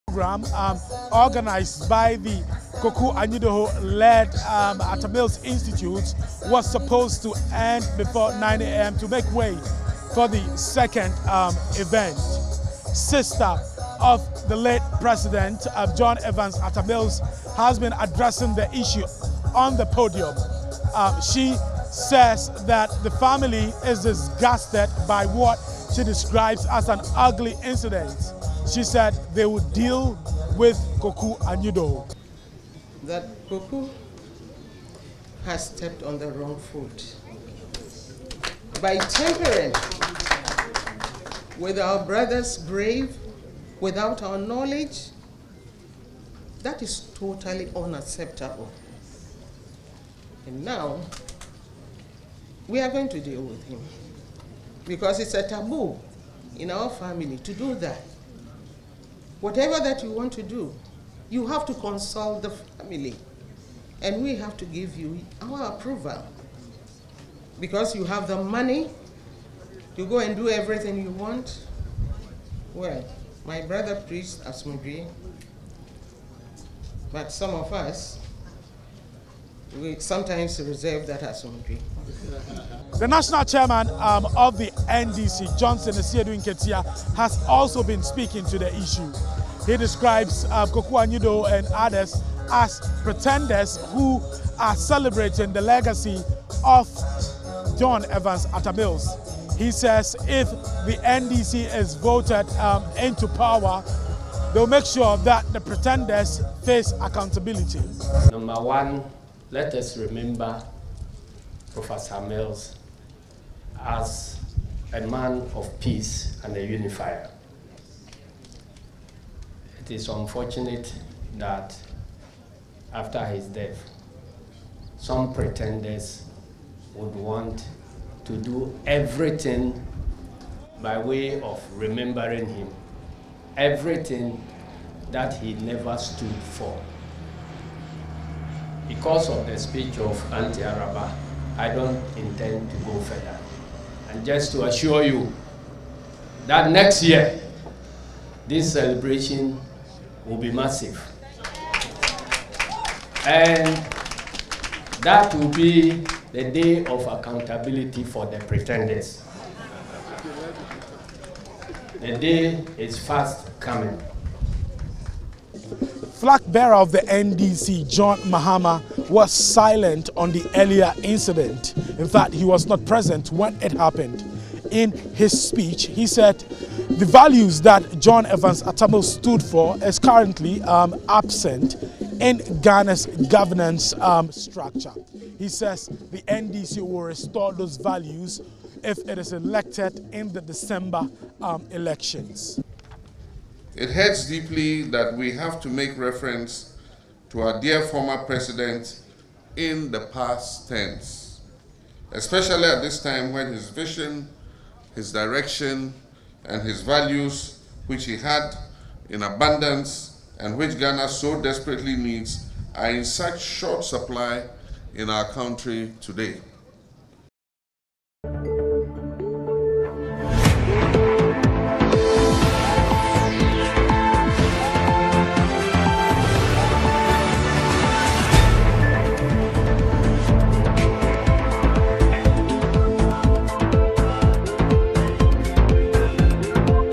Koku Anyidoho clashes with NDC and relatives of Atta Mills at Asomdwee Park.
Atta Mills memorial service: Koku Anyidoho clashes with NDC and relatives of Atta Mills at Asomdwee Park.